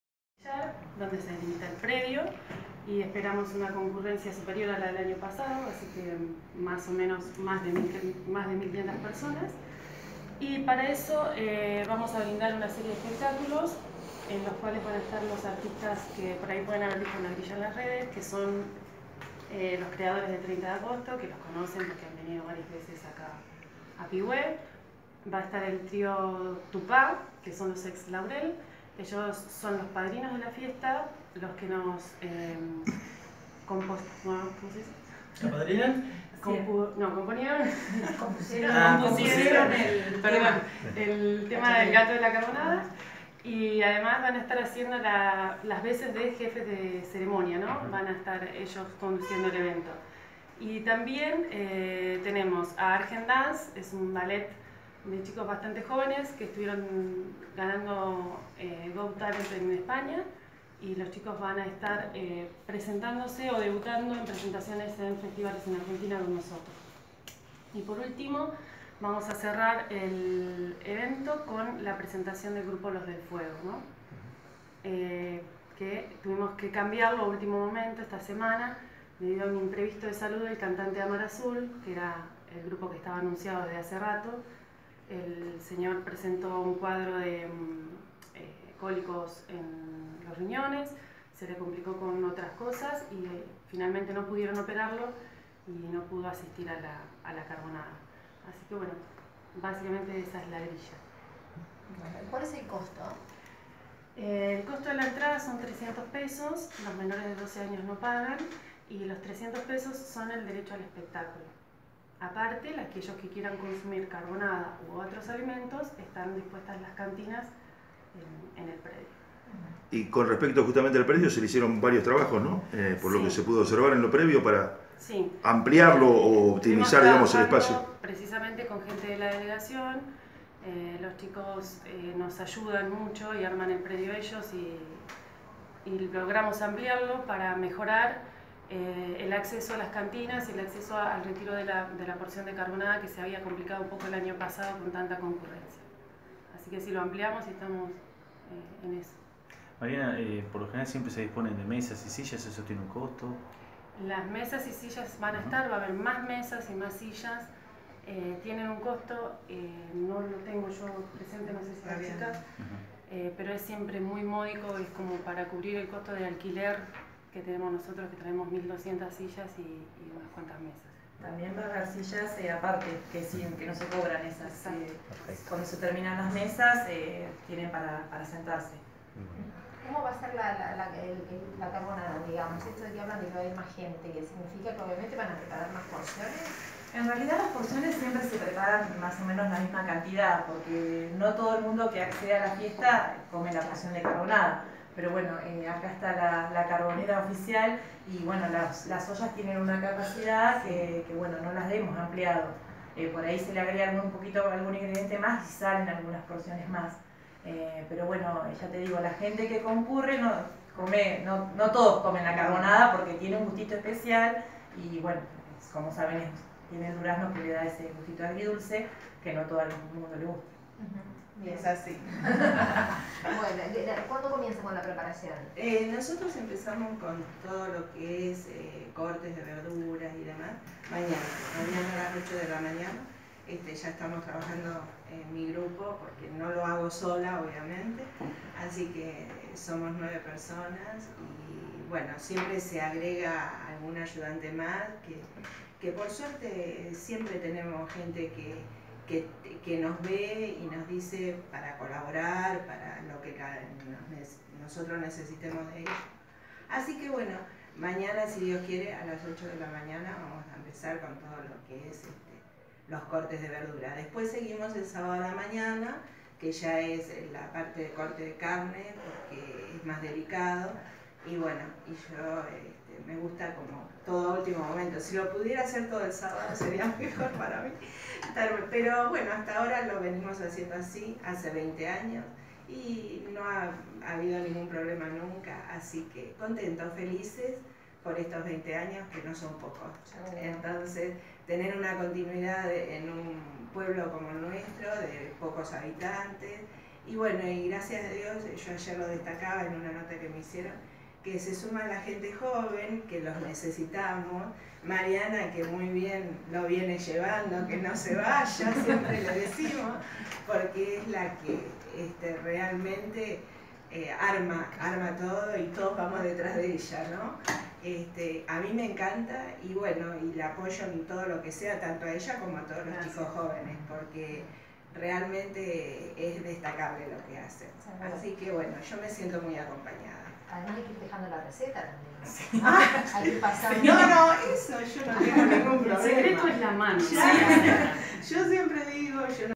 20º Fiesta de La Carbonada de Espartillar
Las integrantes de la Comisión de Cultura de Espartillar Mariana Pronsatti y Lorena Canitrot y Carmen Trech responsable de la elaboración la carbonada presentaron lo que será la vigésima  edición de esa fiesta espartillense .- Lo anunciaron en la sede de cultura municipal acompañados del secretario Agustín Camandona  quien destacó que  desde siempre el evento se autogestiona, puntualizando que la municipalidad solo colabora para su realización.-  Las mujeres espartillenses  en cuanto a los espectáculos recordaron que éste año actuaránel Grupo Tupá (son los ex Trío Laurel los padrinos de la fiesta )  que además serán maestros de ceremonias; danzas Argendance que debuta en Argentina tras actuaciones en España;  los Creadores de 30 de Agosto, y el cierre  de la fiesta con Los del fuego, quienes reemplazan a Amar Azul que por un problema de salud de su cantante no concurren.-La esmerada atencion  que brindarán para quienes deseen adquirir las porciones de carbonada  asi como también otros platos que forman parte de la oferta gastronómica de la fiesta éste sabado 15 de febrero ´20, a partir de las 20:00 hs., en Espartillar.-